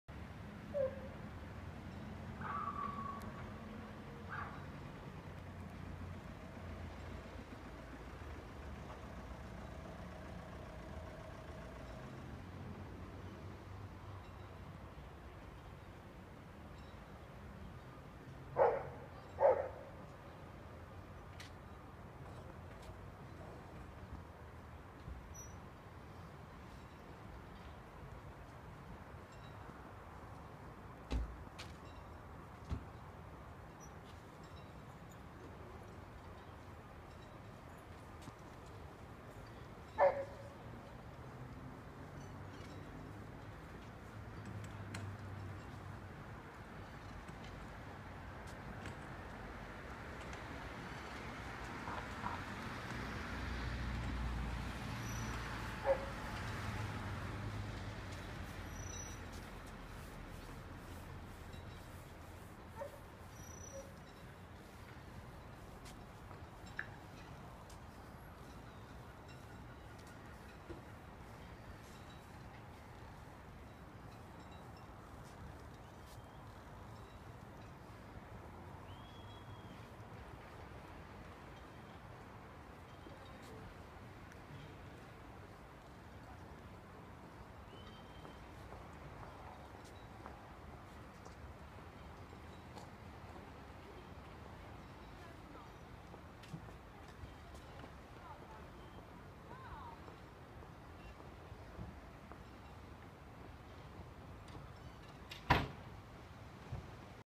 IMPERIA PORTO MAURIZIO: dal porto 360gradi di meraviglia
scenario solo per poche persone,un cane e un'auto lontana